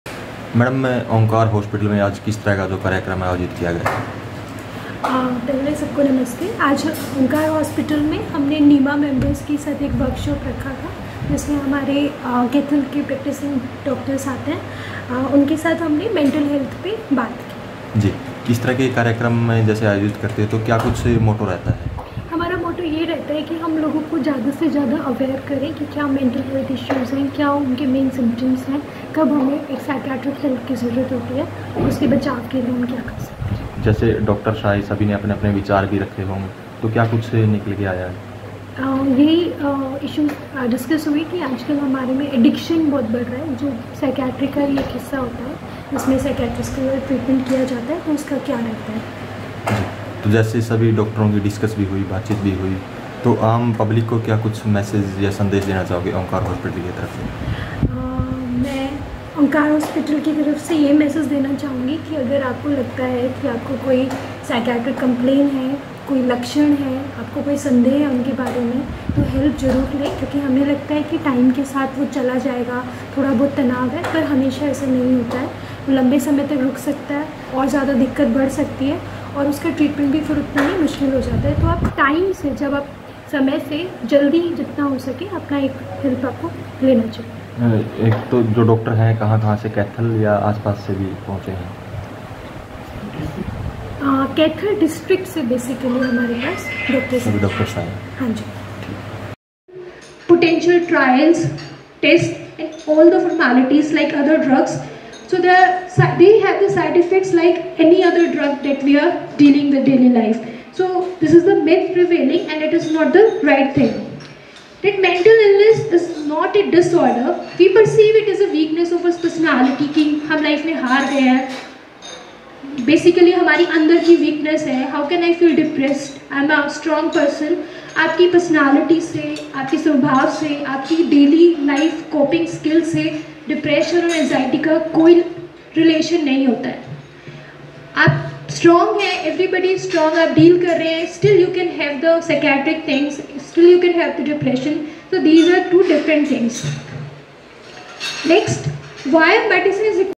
मेंटल हेल्थ के बारे में जागरूकता जरूरी : डा. शिवानी
-कैथल ओंकार अस्पताल की तरफ से लगाया गया कैंप